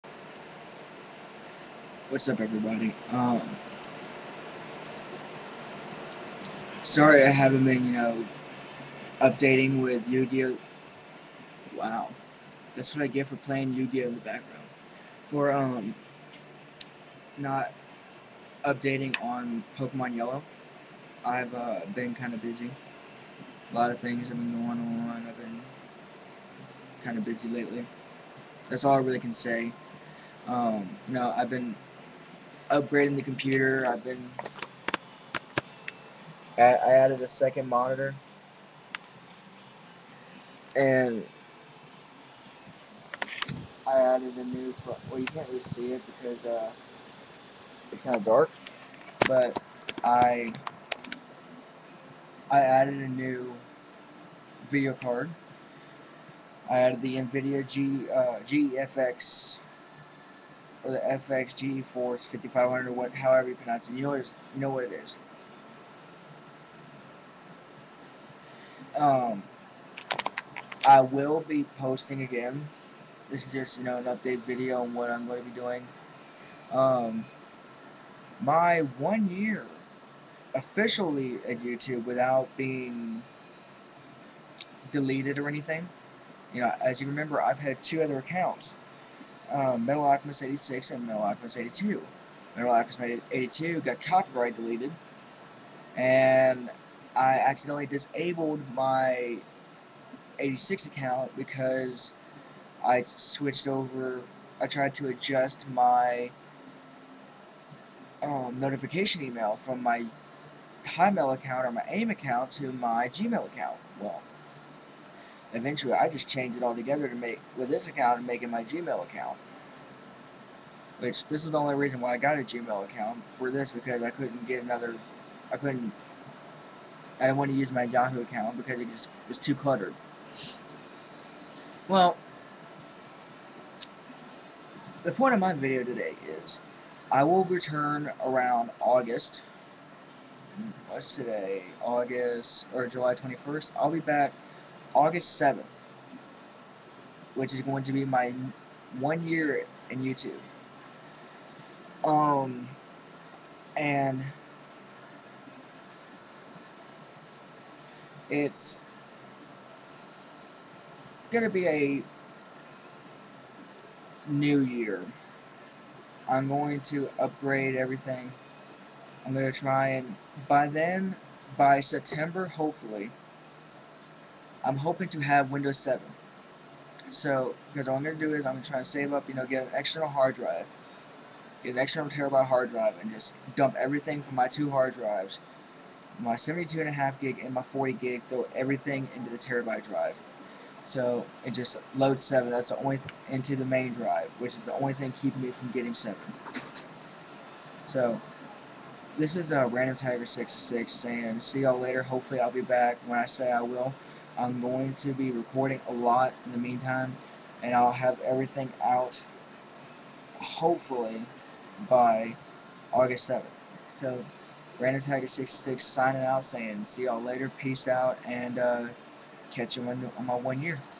I'll return august 7th for my One Year here
that is correct, believe it or not, i've been around here for a year. to be more precise it's actually my 4th year with an account but the other accounts got either disabled or suspended. but i digress